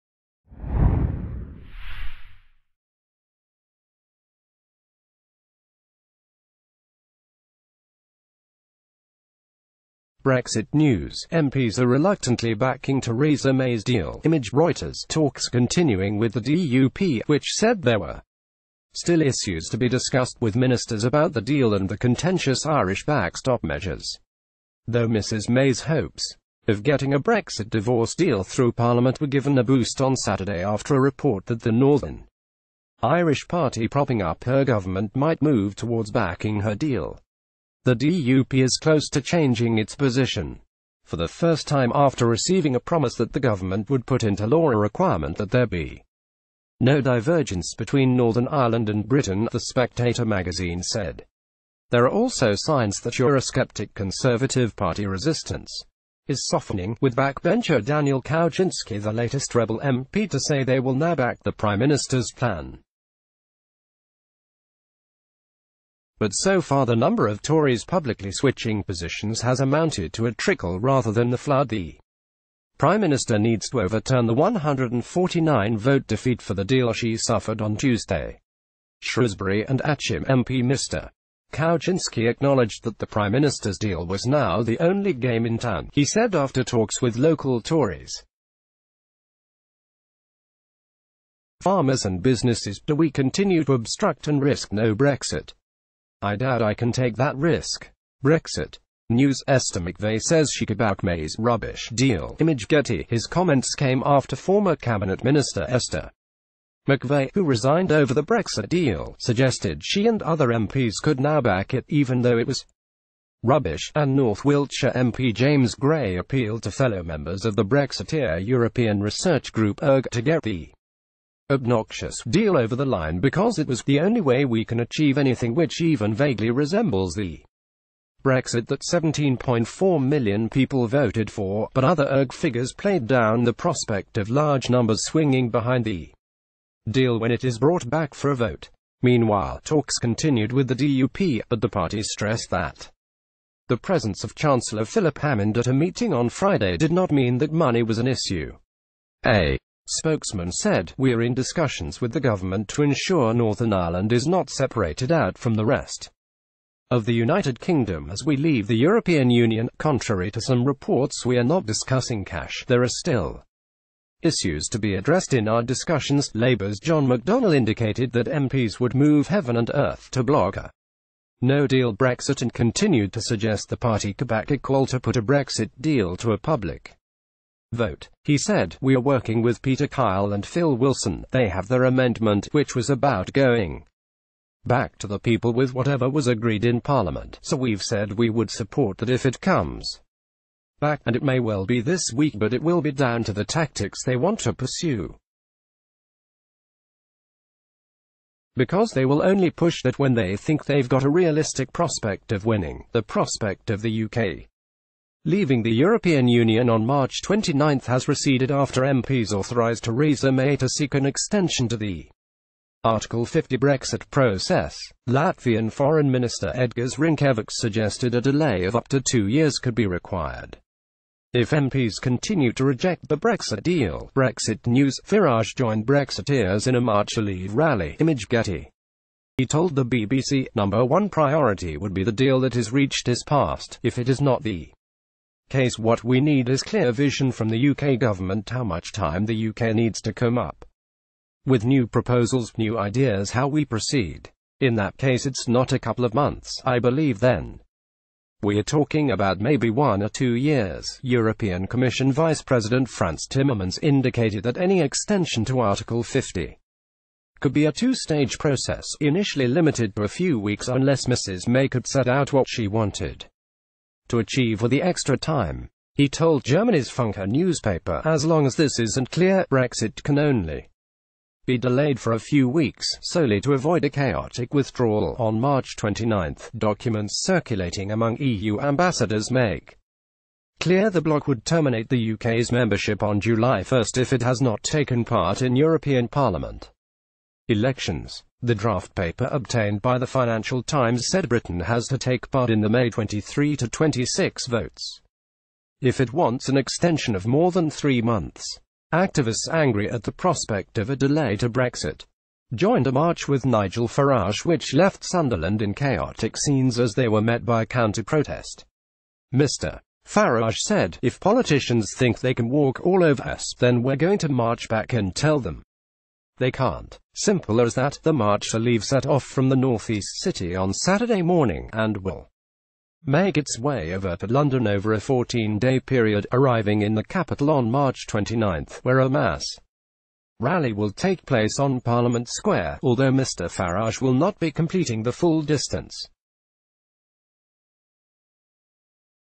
Brexit CHAOS triggers reluctant backing for Theresa May's deal - 'Only game in town'
Brexit news: MPs are reluctantly backing Theresa May's deal (Image: REUTERS)Talks are continuing with the DUP, which said there were "still issues to be discussed" with ministers about the deal and the contentious Irish backstop measures. Though Mrs May's hopes of getting a Brexit divorce deal through parliament were given a boost on Saturday after a report that the Northern Irish party propping up her government might move towards backing her deal. The DUP is close to changing its position for the first time after receiving a promise that the government would put into law a requirement that there be no divergence between Northern Ireland and Britain, the Spectator magazine said. There are also signs that Eurosceptic Conservative Party resistance is softening, with backbencher Daniel Kawczynski the latest rebel MP to say they will now back the Prime Minister's plan. But so far the number of Tories publicly switching positions has amounted to a trickle rather than the flood the Prime Mi...